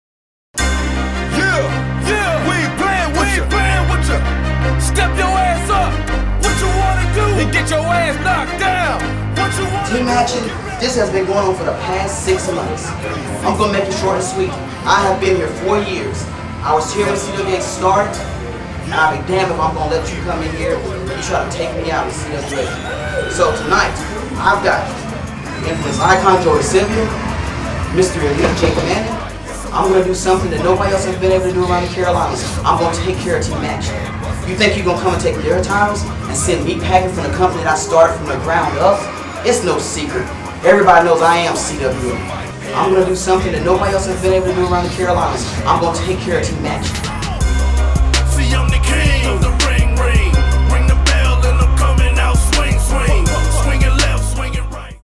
The Elite Icons CWA ROC Promo
CWA presents Rise of a Champion IV with the main event being Team Macktion vs The Elite Icons in a Steel Cage. Sat. Feb 23rd, Grace Park Rec Center in Statesville, NC. Tag team careers on the line.